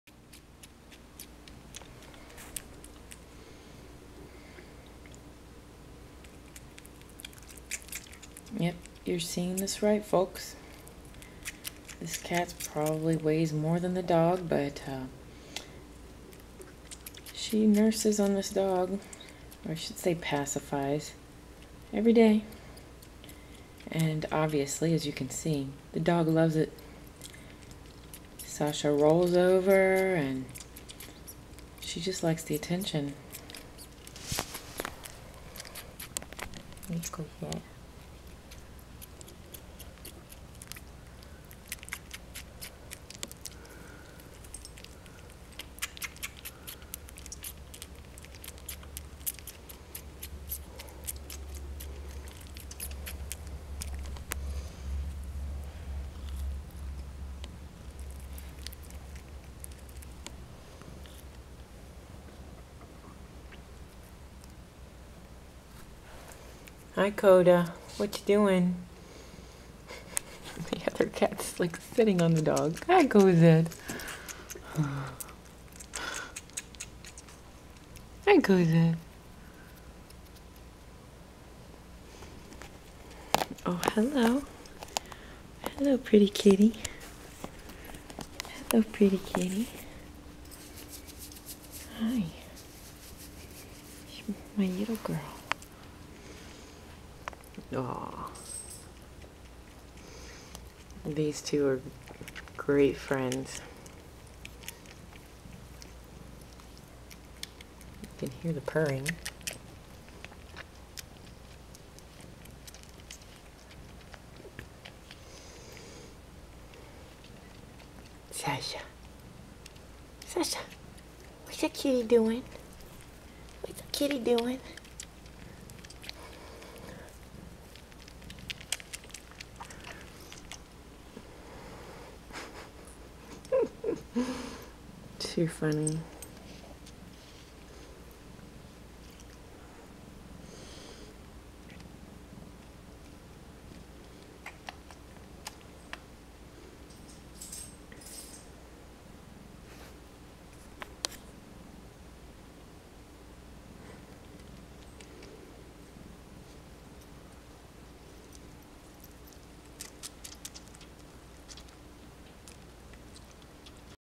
Cat Pacifying on the Dog
This kitten wasn't ready to give up her nursing, so she adapted her habit with our dog.  The funny thing is, the dog likes the attention and even asks her to nurse sometimes.  It's an odd relationship.